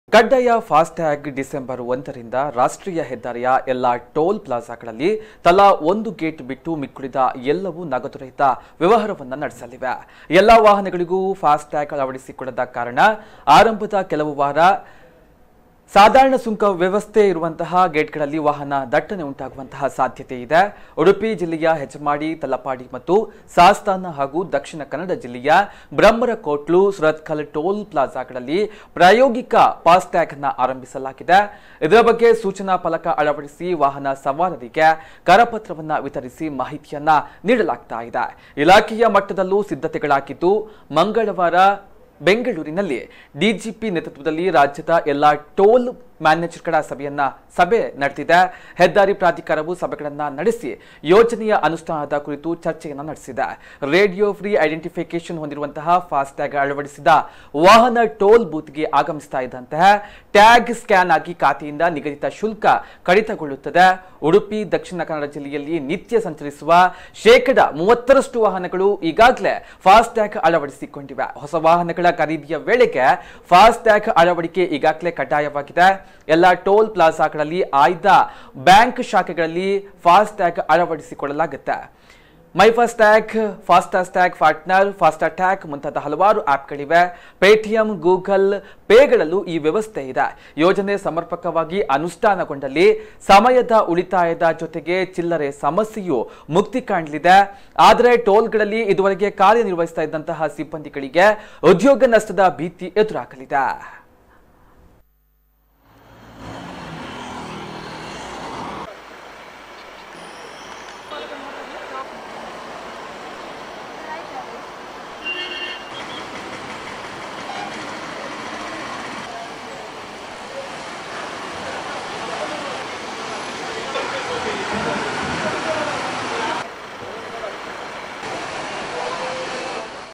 ಡಿ.1ರಿಂದ ವಾಹನಗಳಿಗೆ ಫಾಸ್ಟ್ಯಾಗ್ ಕಡ್ಡಾಯ ಇಲ್ಲದಿದ್ದರೆ ದುಪ್ಪಟ್ಟು ಟೋಲ್..!!!
ಕಡ್ಡಾಯ ಫಾಸ್ಟ್ಯಾ ಗ್‍ನಡಿ ಡಿ. 1ರಿಂದ ರಾಷ್ಟ್ರೀಯ ಹೆದ್ದಾರಿಯ ಎಲ್ಲ ಟೋಲ್ ಪ್ಲಾಝಾಗಳಲ್ಲಿ ತಲಾ ಒಂದು ಗೇಟ್ ಬಿಟ್ಟು ಮಿಕ್ಕುಳಿದ ಎಲ್ಲವೂ ನಗದು ರಹಿತ ವ್ಯವಹಾರ ನಡೆಸಲಿವೆ. ಎಲ್ಲ ವಾಹನಗಳೂ ಫಾಸ್ಟಾಗ್ ಅಳವಡಿಸಿಕೊಳ್ಳದ ಕಾರಣ ಆರಂಭದ ಕೆಲವು ವಾರ ಸಾದಾ ಸುಂಕ ವಸೂಲಿ ವ್ಯವಸ್ಥೆಯಿರುವ ಗೇಟ್‍ಗಳಲ್ಲಿ ವಾಹನ ದಟ್ಟಣೆ ಉಂಟಾಗುವ ಸಾಧ್ಯತೆಯಿದೆ. ಉಡುಪಿ ಜಿಲ್ಲೆಯ ಹೆಜಮಾಡಿ, ತಲಪಾಡಿ ಮತ್ತು ಸಾಸ್ತಾನ ಹಾಗೂ ದ.ಕ. ಜಿಲ್ಲೆಯ ಬ್ರಹ್ಮರಕೂಟ್ಲು, ಸುರತ್ಕಲ್ ಟೋಲ್ ಪ್ಲಾಝಾಗಳಲ್ಲಿ ಪ್ರಾಯೋಗಿಕ ಫಾಸ್ಟಾಗ್ ಆರಂಭಿಸಲಾಗಿದೆ. ಇದರ ಬಗ್ಗೆ  ಸೂಚನಾ ಫಲಕ ಅಳವಡಿಸಿ, ವಾಹನ ಸವಾರರಿಗೆ ಕರಪತ್ರ ವಿತರಿಸಿ ಮಾಹಿತಿ ನೀಡಲಾಗುತ್ತಿದೆ